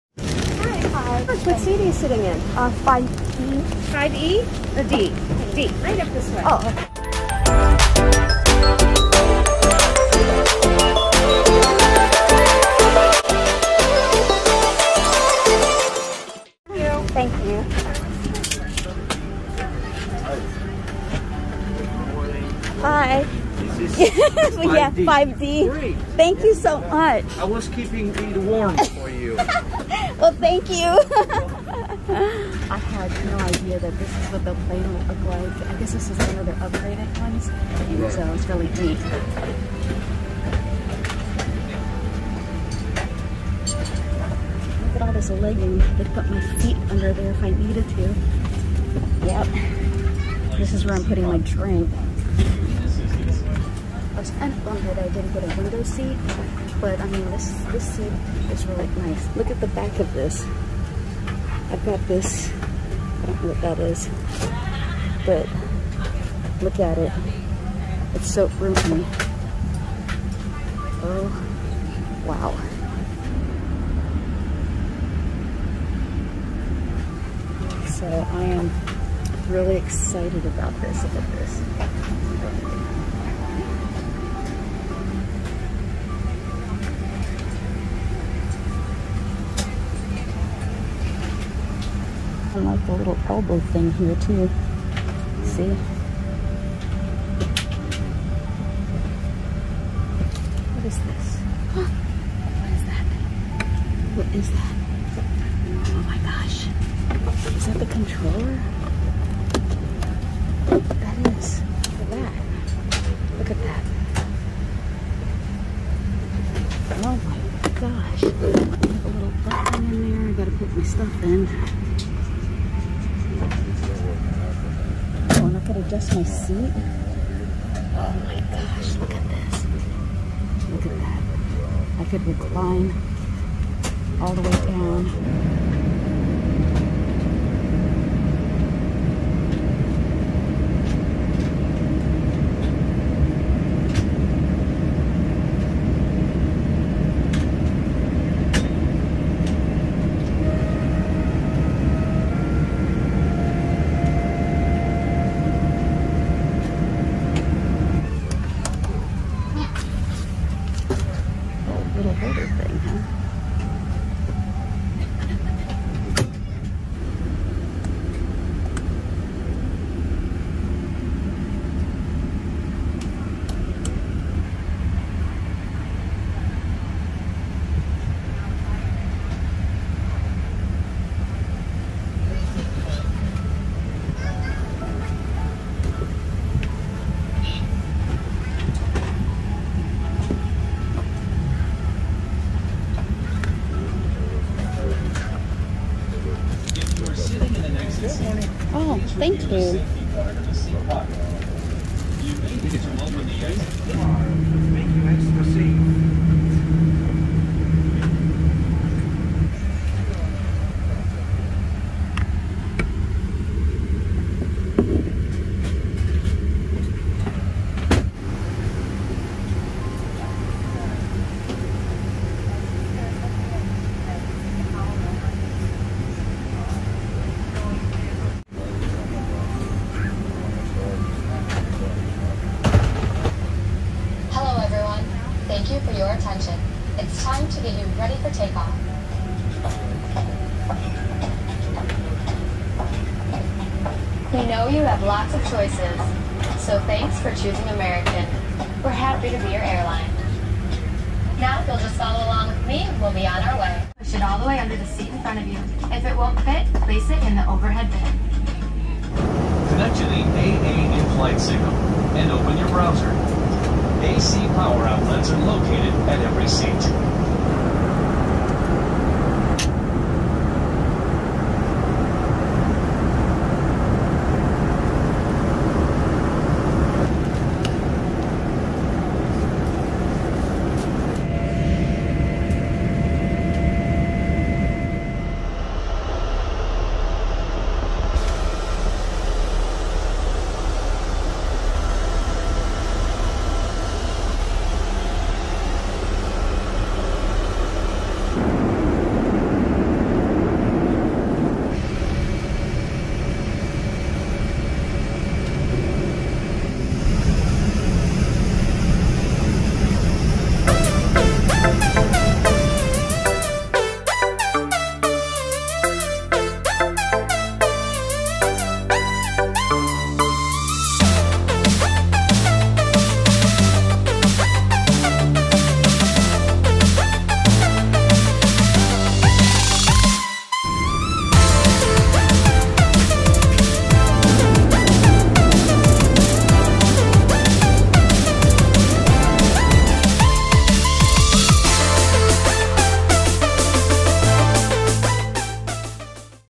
First Time on a Boeing 787-9 Dreamliner (twin-jet)
This was my first time flying in a Boeing 787-9 Dreamliner (twin-jet), business class section!